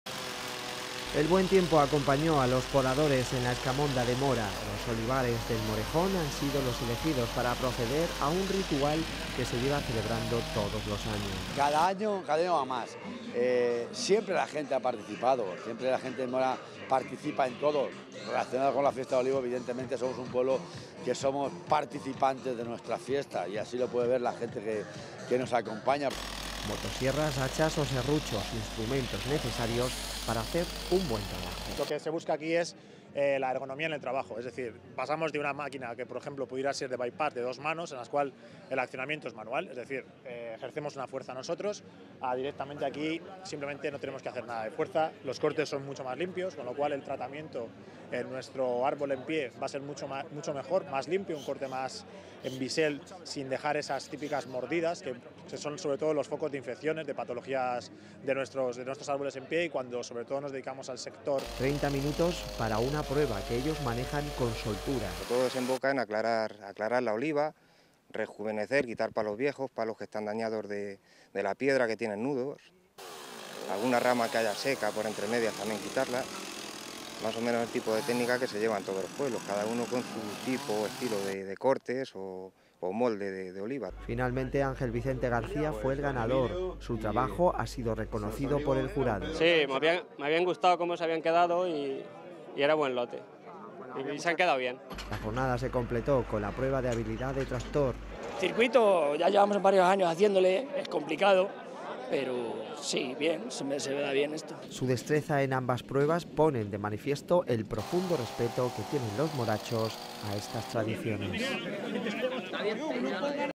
El Concurso de Poda de Olivos vuelve a tener éxito de participación en Mora
Ya son 14 de años de Concurso Regional de Poda de Olivos, una  cita tradicional de los agricultores de Castilla-La Mancha que tiene lugar en la localidad toledana de Mora. Además, junto con él se celebra el Concurso de Habilidad con Tractor.